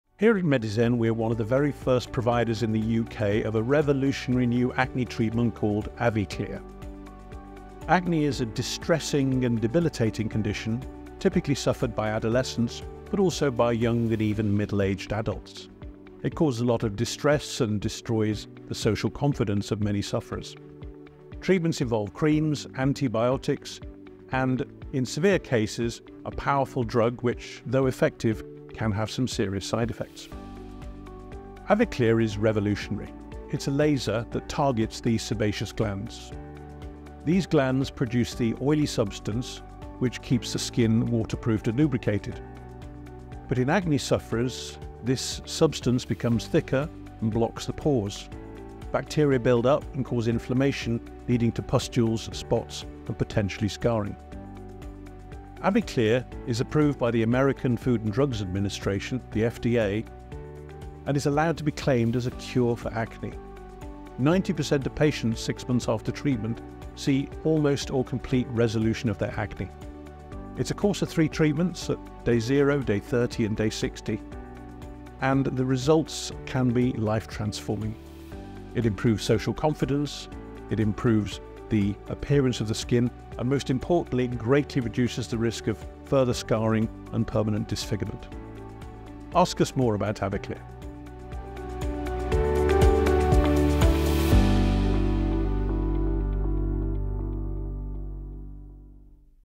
AviClear, the revolutionary laser acne treatment for a future of clear skin ⚡️
AviClear is the first and original FDA-approved laser device for the treatment of mild moderate and severe acne. AviClear uses the power of laser light to selectively target and suppress the sebaceous glands, eliminating acne at the source. Suitable for all skin types, this laser acne treatment provides clear skin quickly and safely. You can achieve clear skin with just three 30-minute treatments!

AviClear is a revolutionary laser which specifically targets the sebaceous glands (where sebum, an oily substance is produced). When too much sebum is produced it can mix with dead skin, clogging pores and therefore resulting in pustules or cysts forming.  After this treatment, your skin will  produce less oil and your acne will begin to improve, helping your skin remain clear long-term.

Alternative acne treatments such as prescription medications target the entire body to treat acne often leaving you with a range of side effects. Because the AviClear laser is specifically targeted at the area with acne, side effects are minimal with little to no downtime.

AviClear is recognised by the FDA as a proven treatment for the long-term clearance of acne. Patients in the clinical trial reported that the results of the treatment got progressively better with time at 12, 18 and 24 months.